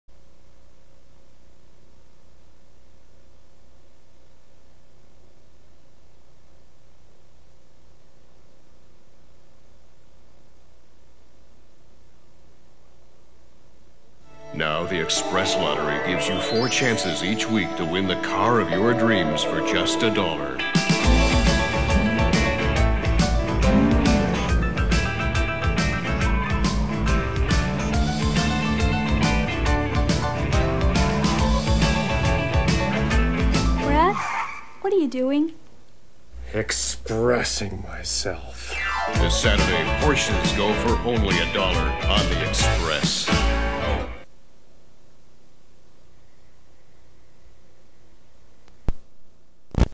The Express Lottery
A commercial made by Cactus Productions for the BC Lottery Corporation in the fall of 1988.   I think this was my first commercial, maybe even my first significant paid gig.  Regardless, it was definitely the first of a series of what I called "hair gigs".

All I remember about this was spending a cold November night in my pajamas pretending to have fun driving around Vancouver in a Jaguar XJ6 that was loaded onto a flatbed trailer.  The lovely actress who played my wife (whose name I'm sorry I don't remember) was very professional and very patient.

The commercial had a strong 13 week run, to the point where I was often recognized but was so pervasive that I was mostly dead to casting directors for the next year.